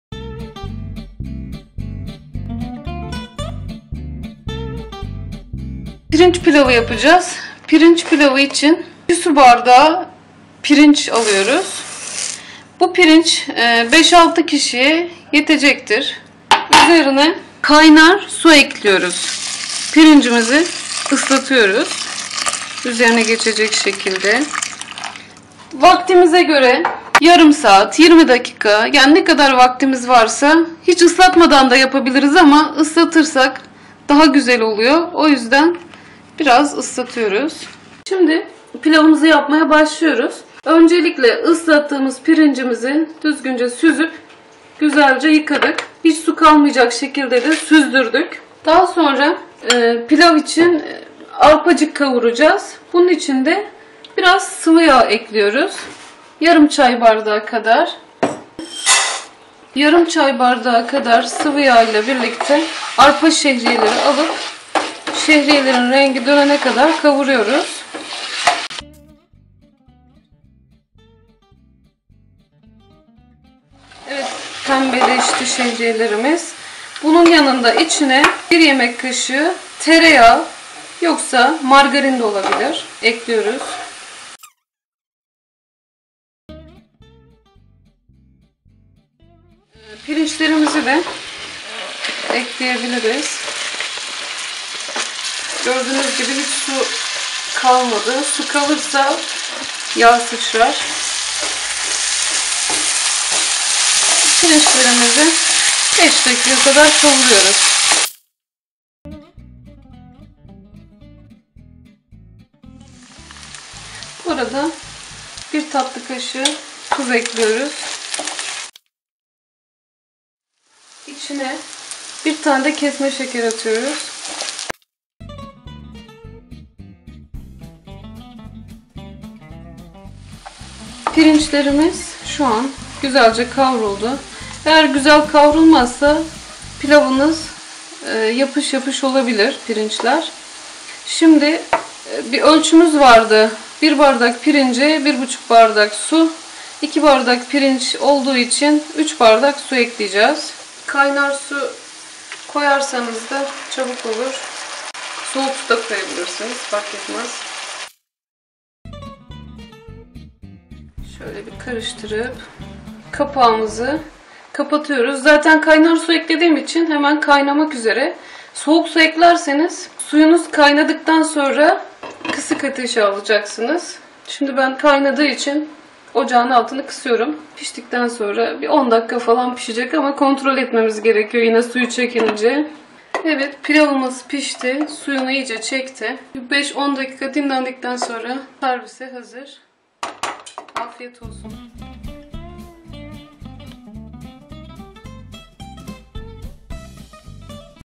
PİRİNÇ PİLAVI TANE TANE
Tane tane pirinç pilavı hazırlamak zor değil. Bu bölümü izleyerek ve anlattığım tarife göre beğenilen, kıvamında, tane tane pirinç pilavı hazırlayabilirsiniz.
Malzemeler
2 su bardağı pirinç (5-6 kişi için)
2 yemek kaşığı arpa şehriye
1 çay bardağı sıvı yağ
1 yemek kaşığı tereyağ veya margarin ( Tereyağ veya margarin kullanmıyorsanız, yarım çay bardağı daha sıvı yağ ilave edin)
Tuz
1 bardak pirince 1,5 bardak su olacak şekilde su.